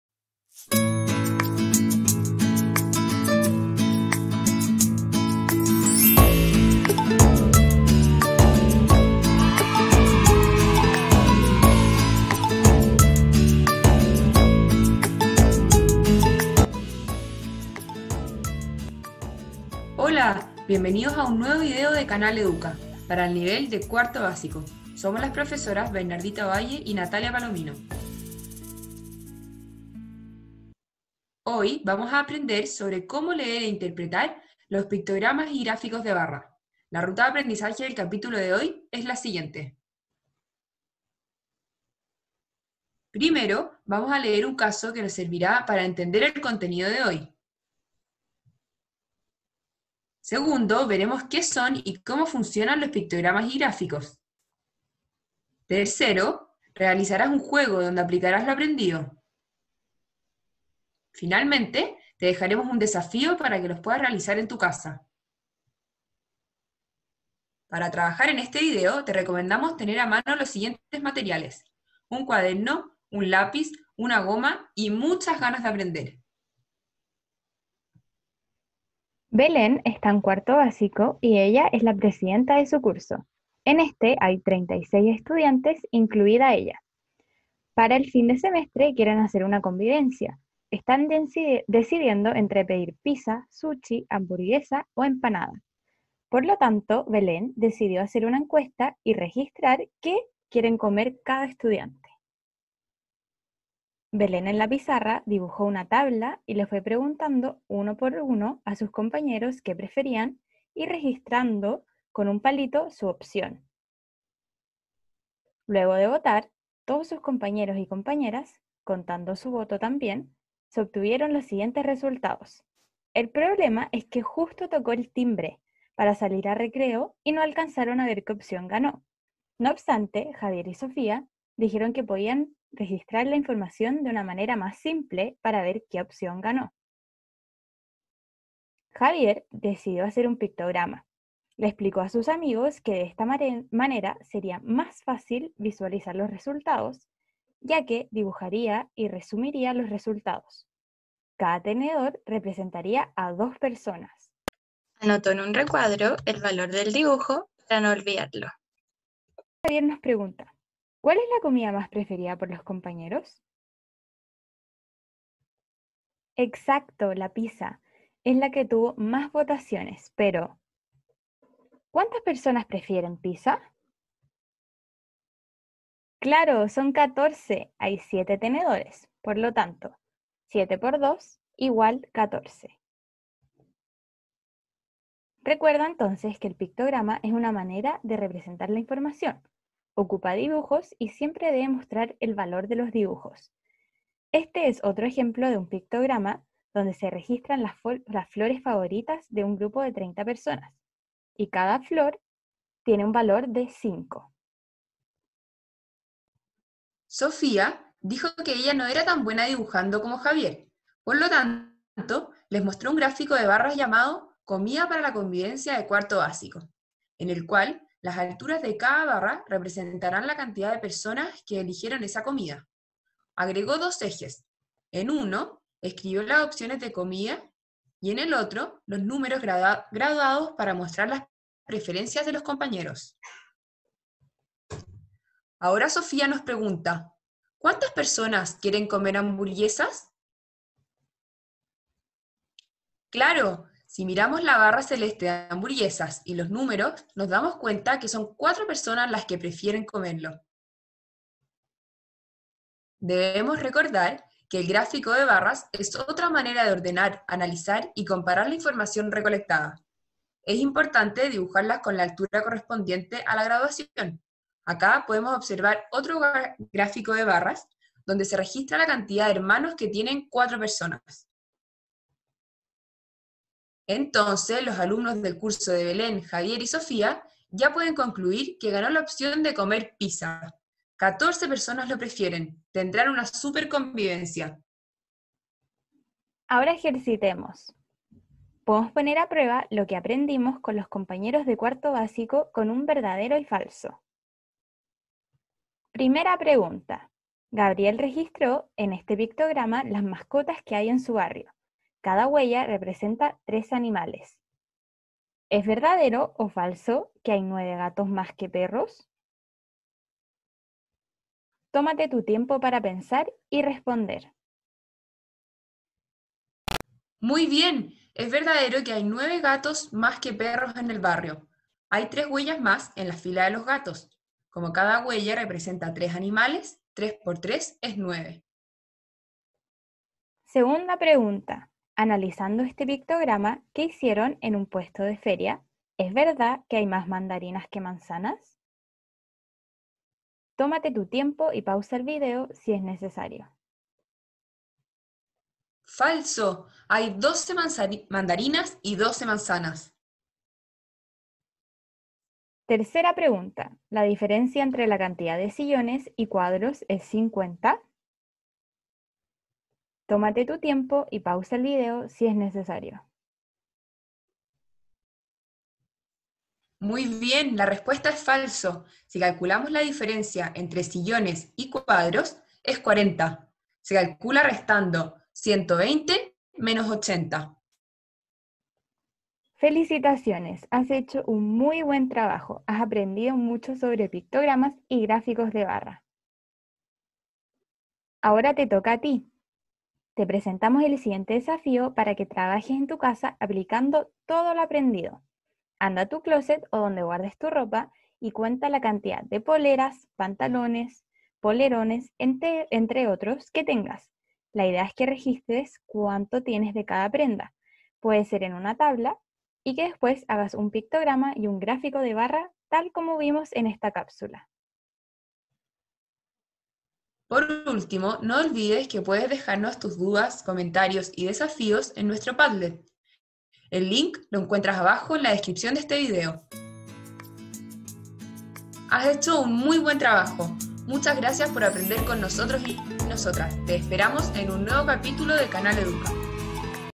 Pictogramas y gráficos de barra - Matemática| 4°Básico
En este vídeo aprenderás a leer e interpretar pictogramas y gráficos de barras.

Asignatura y curso: Matemática, 4°Básico.

OA: OA27: Leer e interpretar pictogramas y gráficos de barra simple con escala, y comunicar sus conclusiones.

Google Imágenes